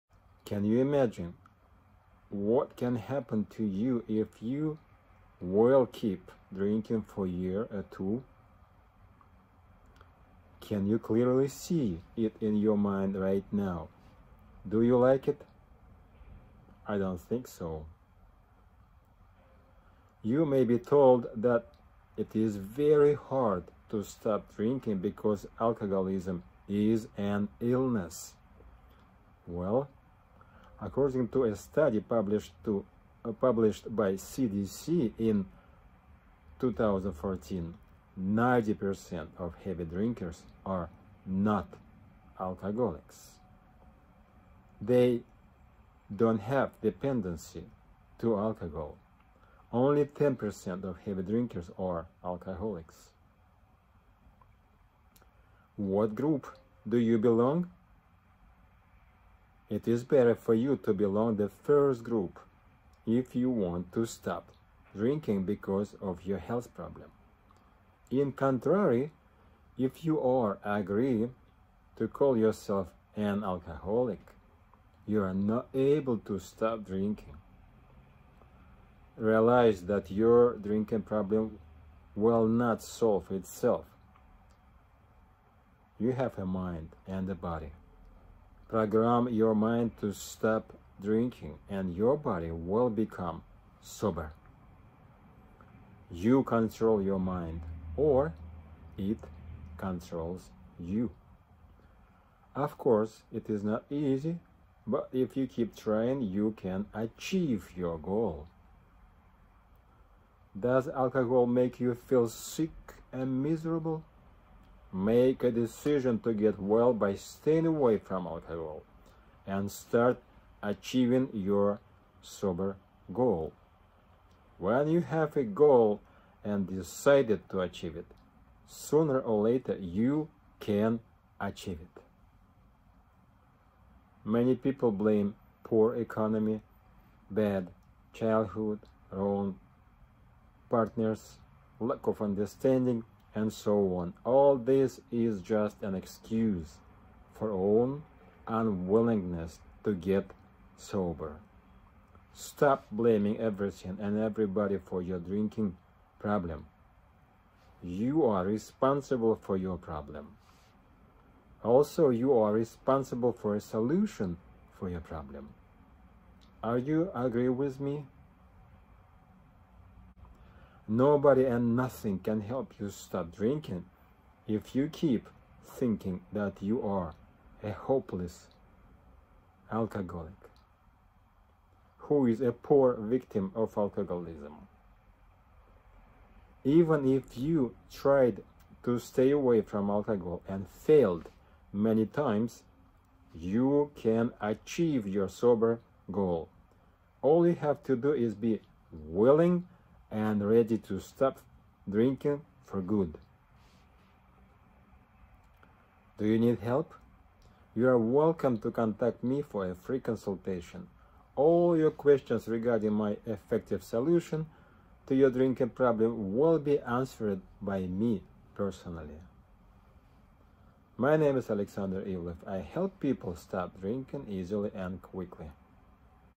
You Can Achieve Your Sober Goal!
Your Drinking Problem Can Be Solved Without Rehab, Psychotherapy, AA, or Pills.

But you have to be WILLING to give up your drinking habit!

If you enjoy alcohol, and feel good after drinking, then you will resist any  help designed to reduce or eliminate your urge to drink. Right?

Only if you already try to stay sober, because you have an alcohol-related serious health problem, you can become a sober person who enjoys being healthy.

My name is Alexander Ivlev. As a certified hypnotist, I help people stop drinking for good easily and quickly.